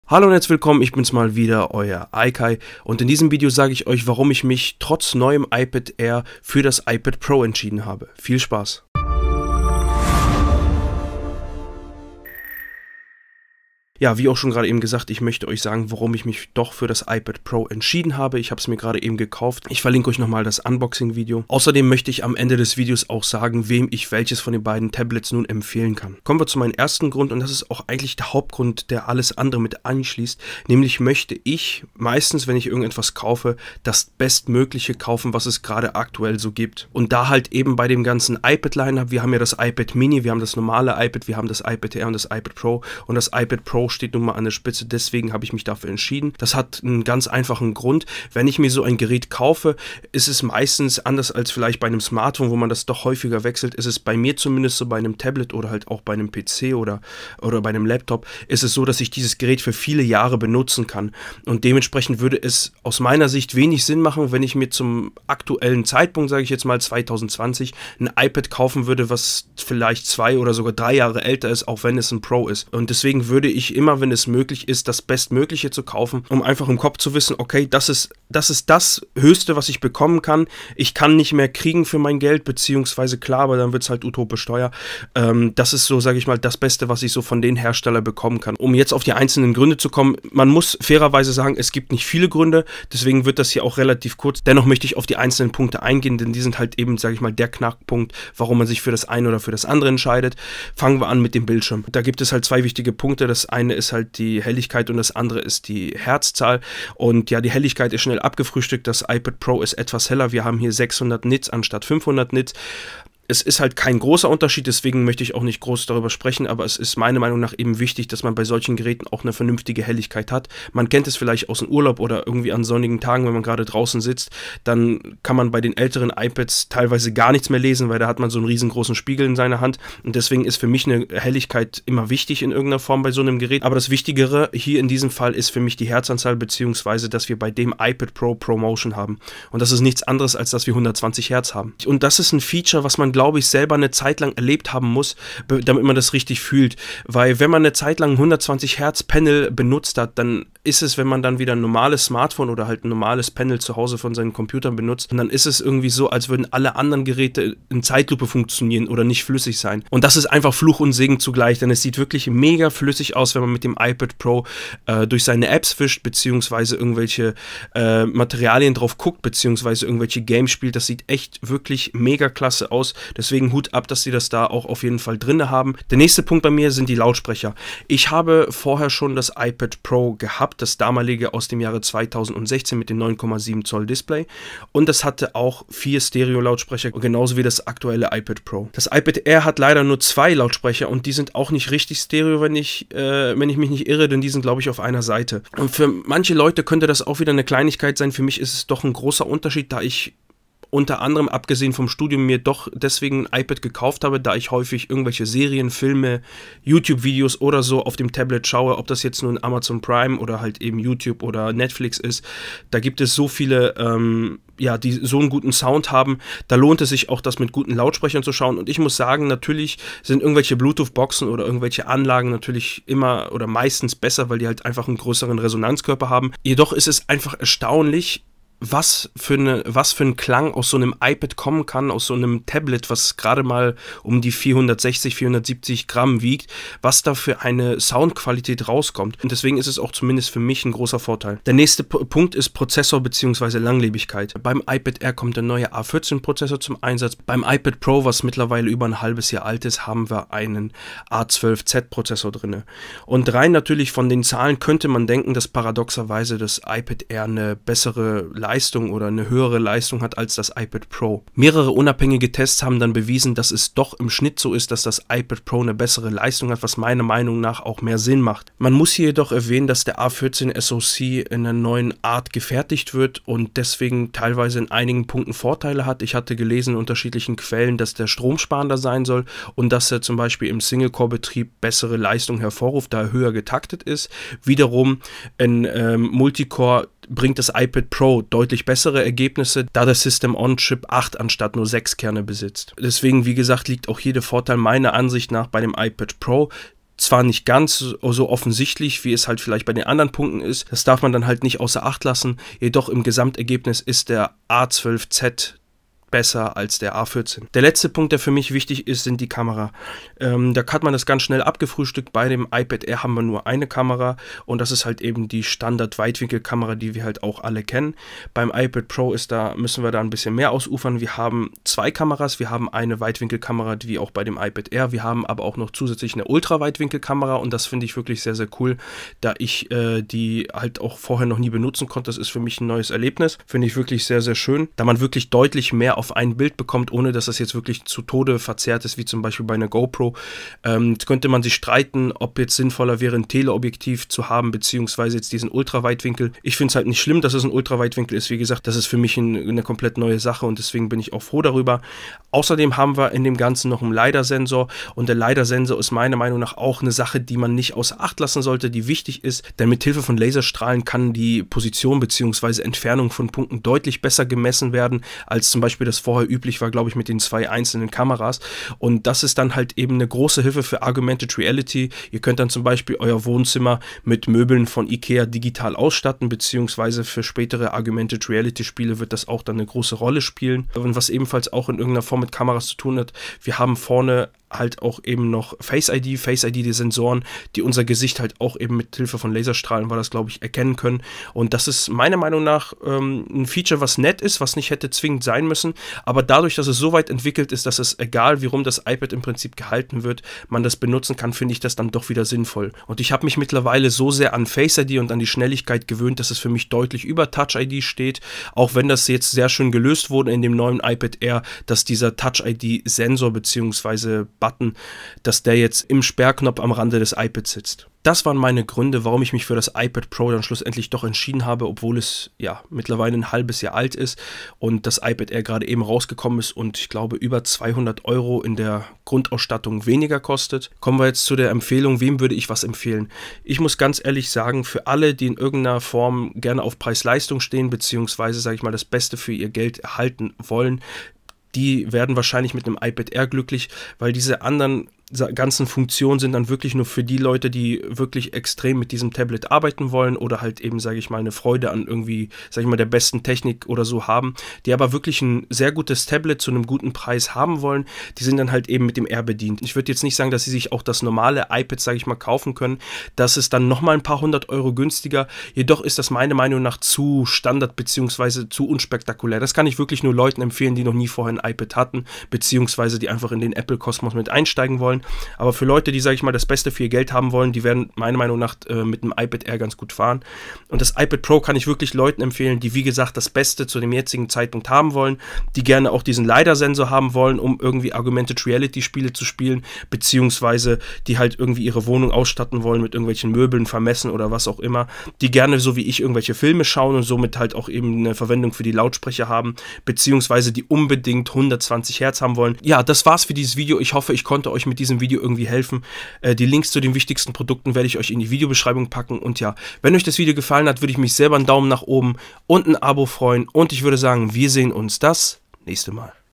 Warum ich mich für das iPad Pro entschieden habe + Kaufempfehlung (Deutsch)
Hallo,
in diesem Video nenne ich euch meine Gründe, warum ich mich für das iPad Pro und nicht für das iPad Air entschieden habe.
Viel Spaß

Inhaltsverzeichnis:
00:00 Einleitung
00:26 Der Hauptgrund
01:28 Der Bildschirm
02:55 Die Lautsprecher
04:01 Der Prozessor
05:07 Die Kameras
07:10 Kaufempfehlung: Air oder Pro
08:22 Fazit

MacBook Pro 13" 2015:
Intel Iris Graphics 6100
16GB Arbeitsspeicher
1TB SSD